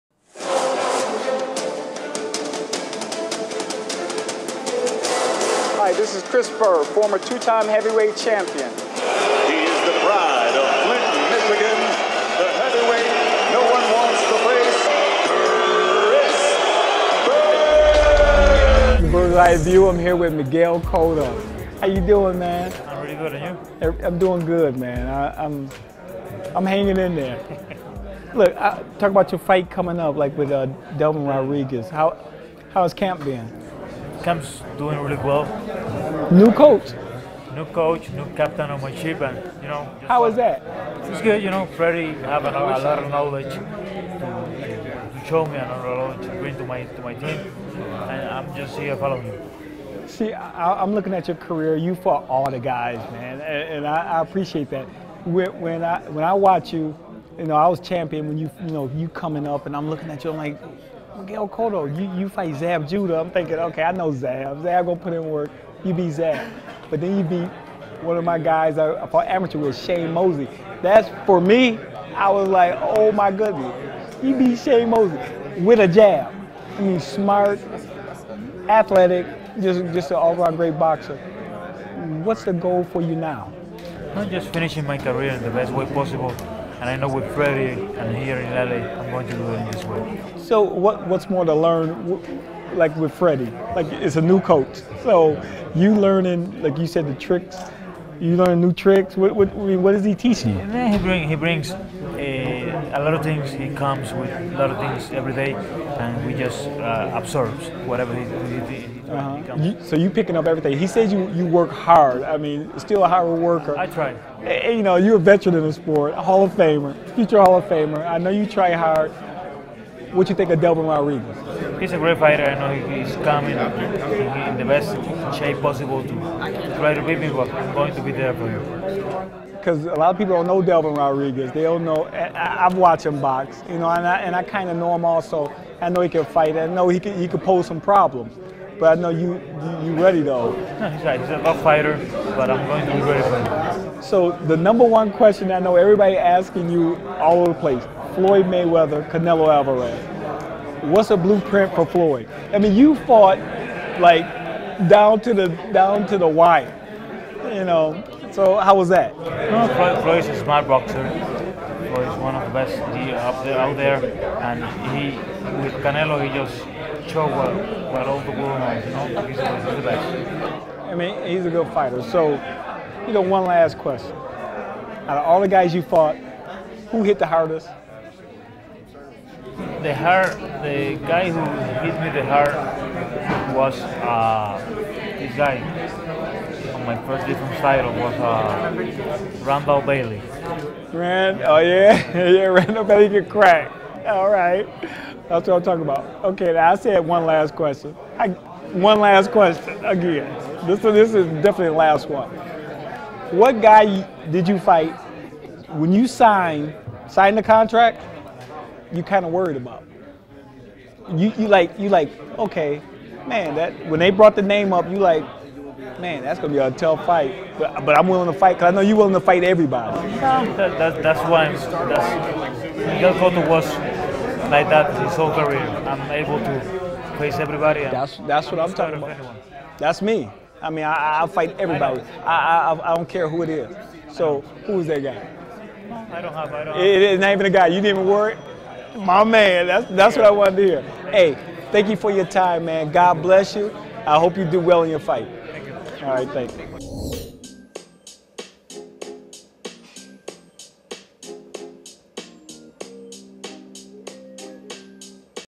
The Byrd's Eye View - Miguel Cotto - Episode 34
Chris Byrd catches up with Miguel Cotto in his training camp and media day at the Wild Card Gym in Los Angeles, CA. Cotto a former WBA light middleweight champion is looking to gain a shot at the title again.